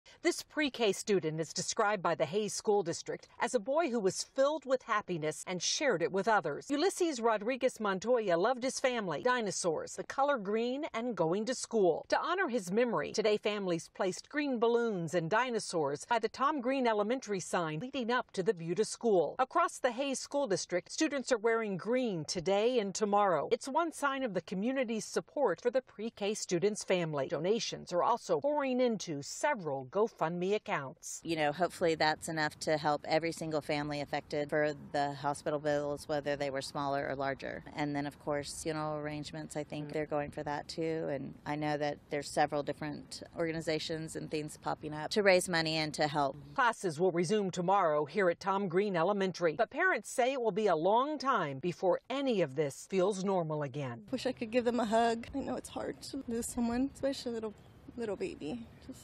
Hays CISD community honors child crash victim with dinosaur tributes, green balloons
Hays CISD students, teachers, and parents are doing what they can to support the family of Ulises Rodriguez Montoya and other students injured in the fatal rollover crash from last Friday.